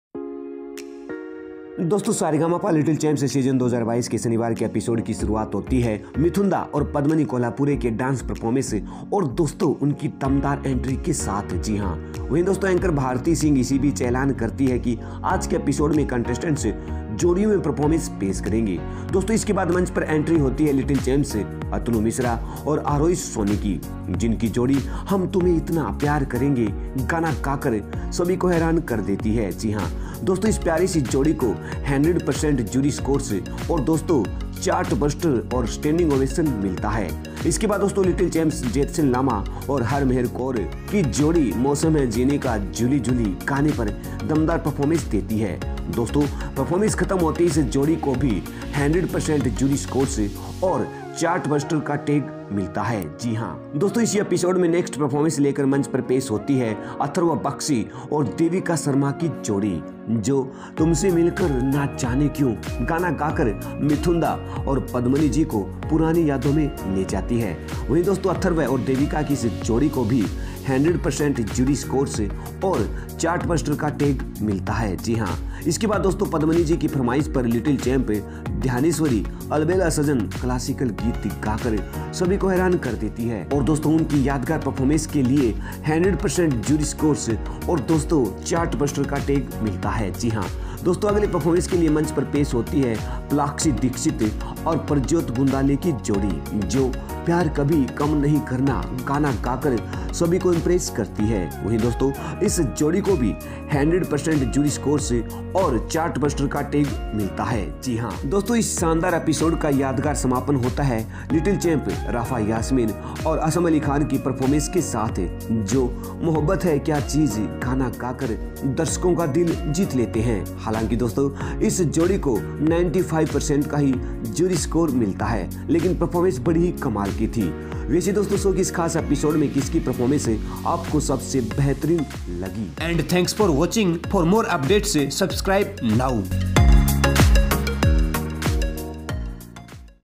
Saregamapa Lil Champs 2022 | Episode 9 | Full Review | Kiski Performance Ne Jeeta Juri Ka Dil
Saregamapa Lil Champs 2022 | Episode 9 | Full Review | Kiski Performance Ne Jeeta Juri Ka Dil | 12 November 2022 | Sat | Mithun Padmini kolhapure Special Episode.

Dekhiye SRGMPLilChamps2022 15th October se, Sat-Sun, raat 9 baje, sirf ZeeTV par.